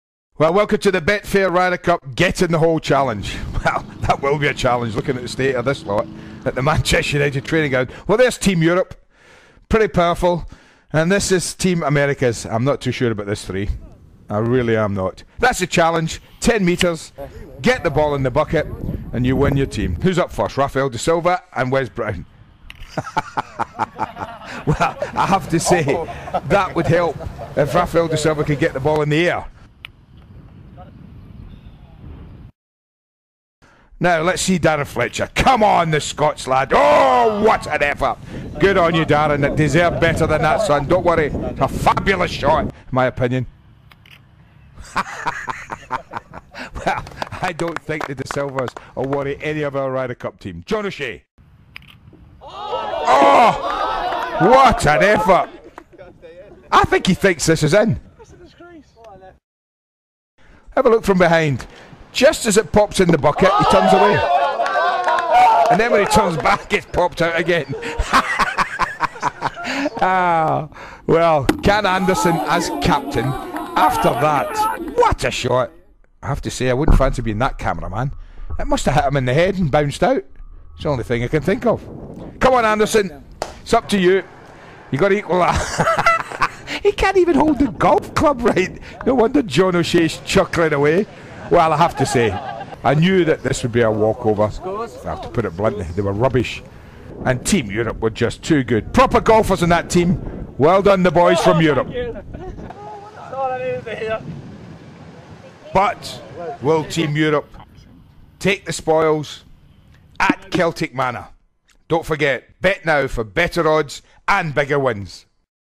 Golf
Ryder Cup get in the hole challenge with ManU -- Team Europe vsTeam America at a 10m challenge, get the ball in the bucket.